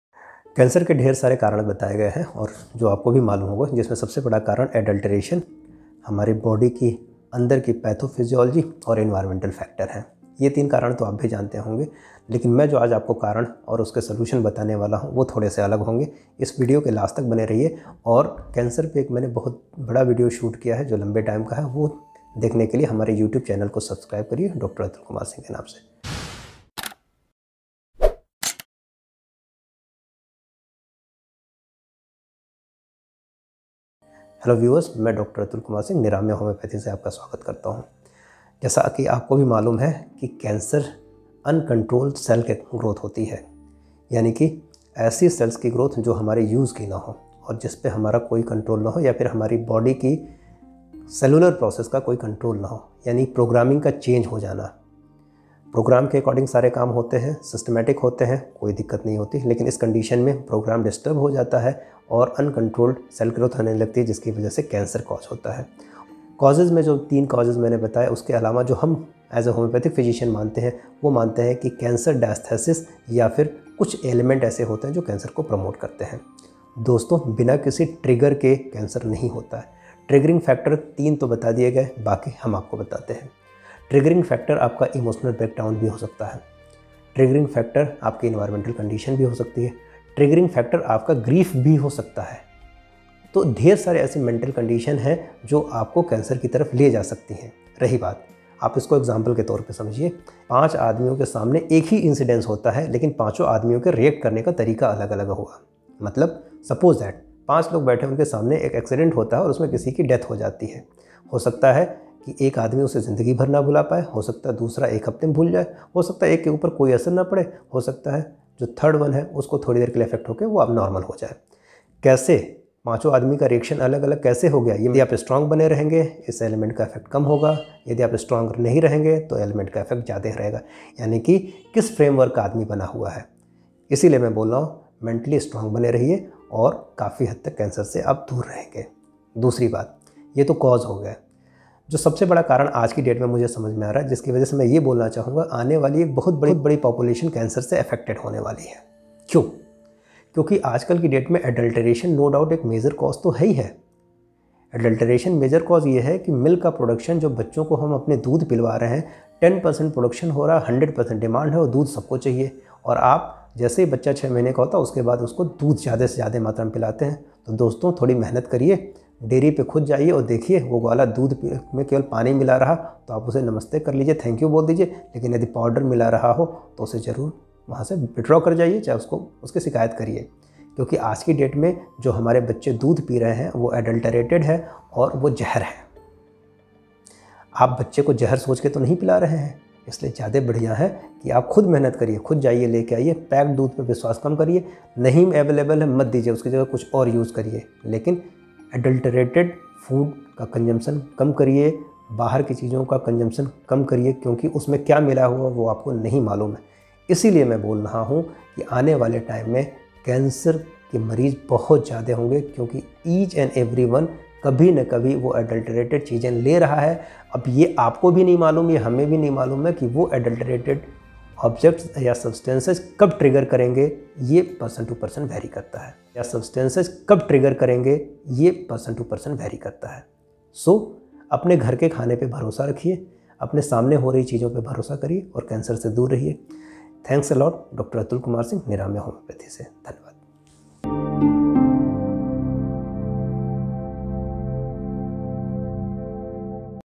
Cancer precautions | Cancer treatment | How to save yourself from cancer | Avoid regrowth of cancer
Precautions with early diagnosis and early treatment is main procedures to adopt for prevention and control of cancer.